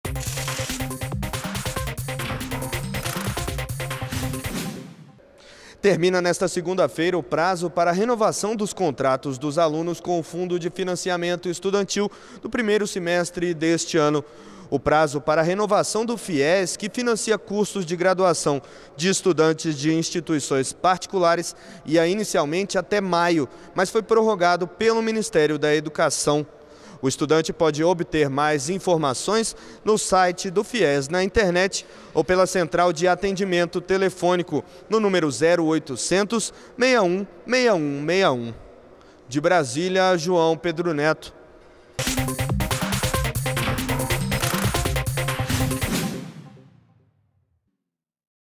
Termina hoje o prazo para renovação de contratos do Fies
ÚLTIMAS  - 20.07.15: O prazo para renovação de contratos do Fundo de Financiamento Estudantil (Fies) termina nesta segunda-feira (20). Alunos graduação podem solicitar o financiamento dos estudos em instituições particulares e quitar os débitos apenas após a conclusão do curso. detalhes em mec.gov.br